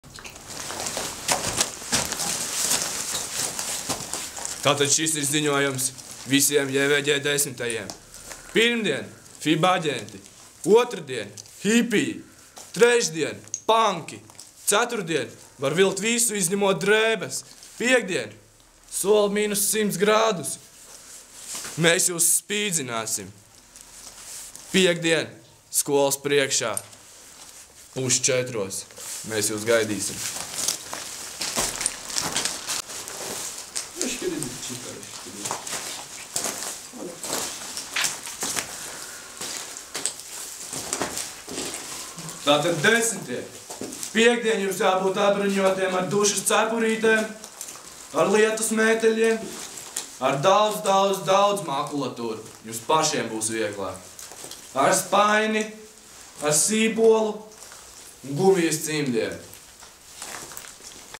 JVĢ iesvētību ziņojums 10.klasēm (2012)
Filmēja -  Klaids Lielbiksis
Ziņojums Jūrmalas Valsts ģimnāzijas 10.klasēm:

Iesvētību nedēļā jāģērbjas:
PIRMDIEN : FIB AĢENTI
OTRDIEN : HIPIJI
TREŠDIEN : PANKI
CETURTDIEN : VAR VILKT VISU, IZŅEMOT DRĒBES
PIEKTDIEN: ĢĒRBTIES ATTIECĪGI, JA ĀRĀ IR -100° !

Kas jāņem līdzi uz iesvētībām?
Dušas cepurīte, lietusmētelis, daudz žurnāli & avīzes (vieglāk ies iesvētībās), spainis, sīpols, gumijas cimdi!